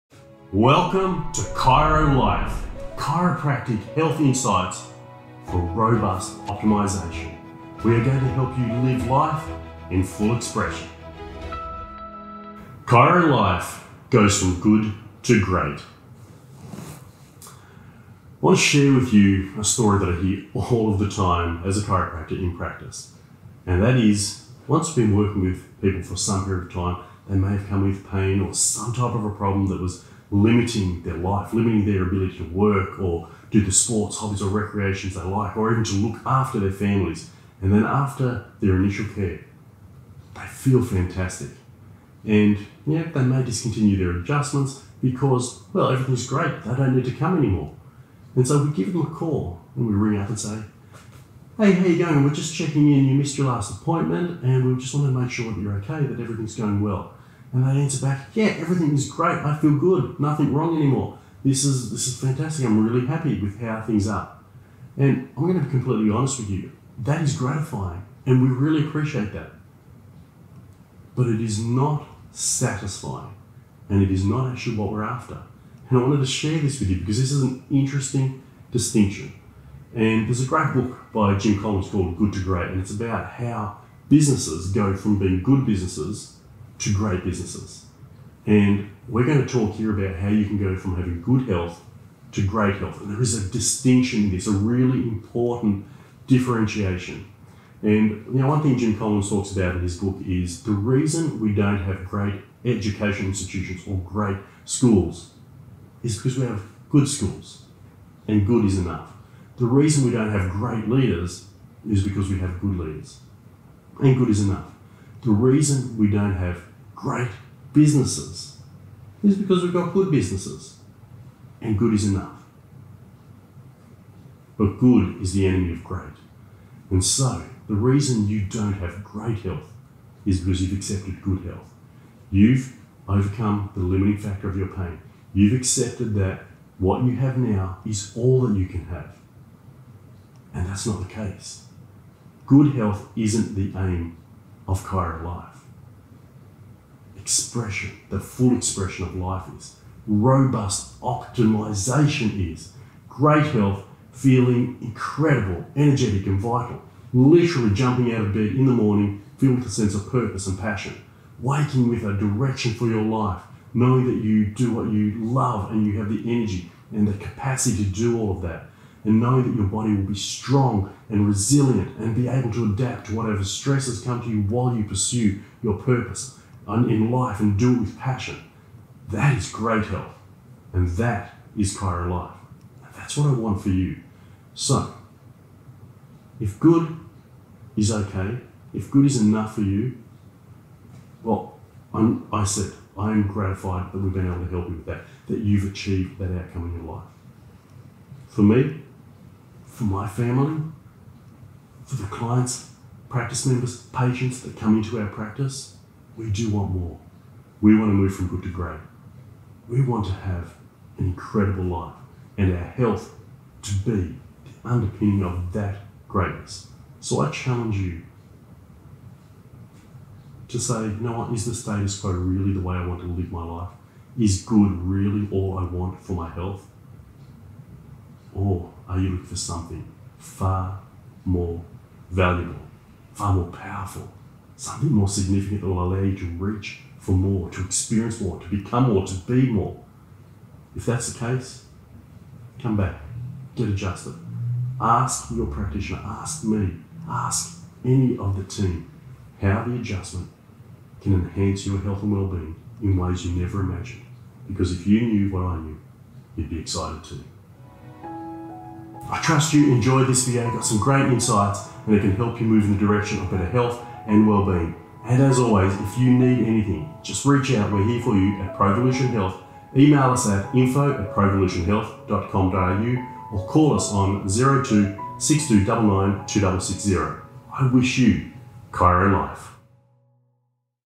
Chiro Life Goes From Good To Great
🎁 About Chiro Life
There are many reasons why clients may choose not to reschedule another appointment in their chiropractic journey. The purpose of this ChiroLife series is to address the questions and concerns in regards to the Chiropractic Journey that have been raised when asked of our clients. The questions addressed in this series are ones that were found most common within our research.

💌 Marcus Chacos answers today’s question: "Can I ever miss an adjustment?"

Keep up with Marcus Chacos and his practice at his Facebook